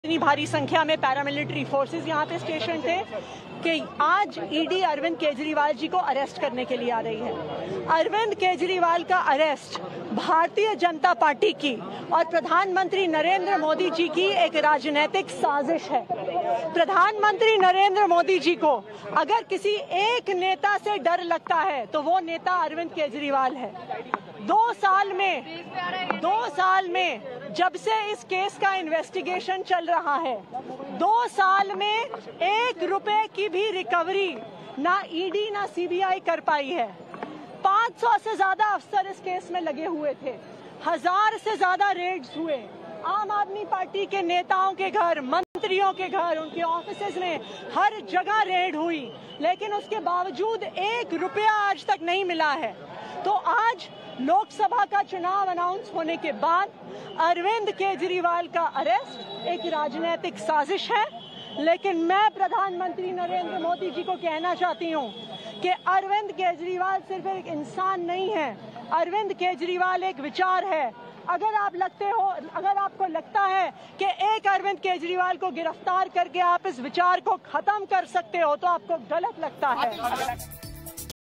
Arvind Kejriwal Arrested: 'Kejriwal's Arrest Is Political Conspiracy Of BJP...' Says AAP Min Atishi
ED arrested Delhi CM Arvind Kejriwal in the liguorgate scam case. "Kejriwal's arrest is the political conspiracy of the BJP and PM Modi," said AAP Minister Atishi. Watch In!